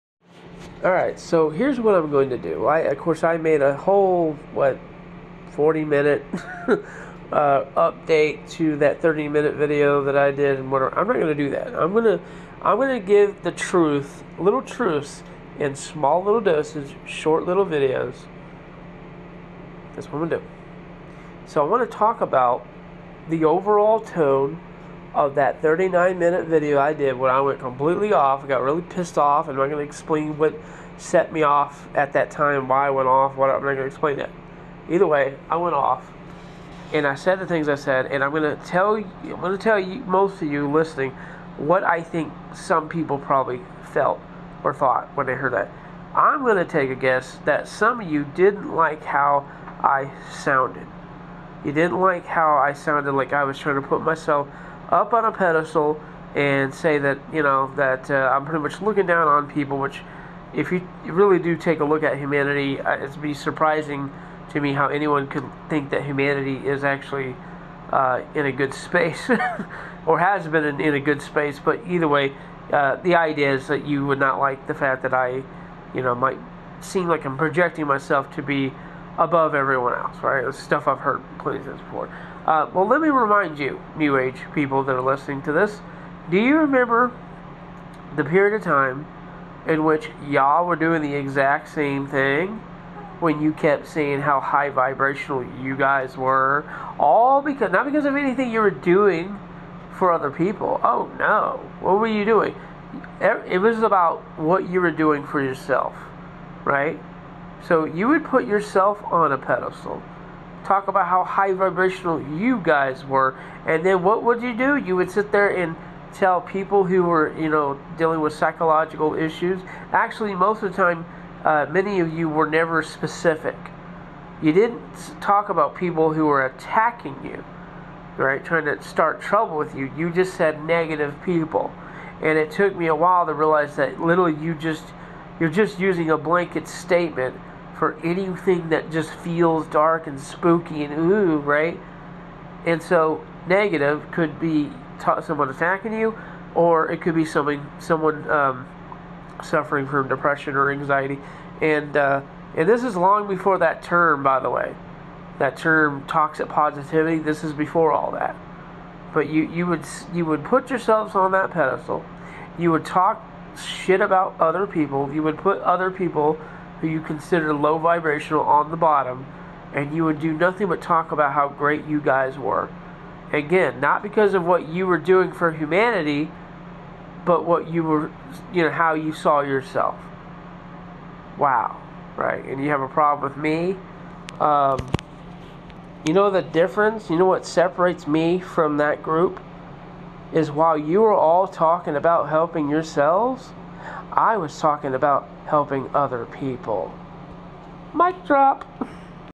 Truth Dose #1
Dr Silver is here to give you your truth medication. instead of overmedicating you like I might have done before, I decided to reduce your intake with just one truth pill at a time. I've got only one problem though, I need a hot nurse assistant to help me with all of these patients, does anyone know where she's at?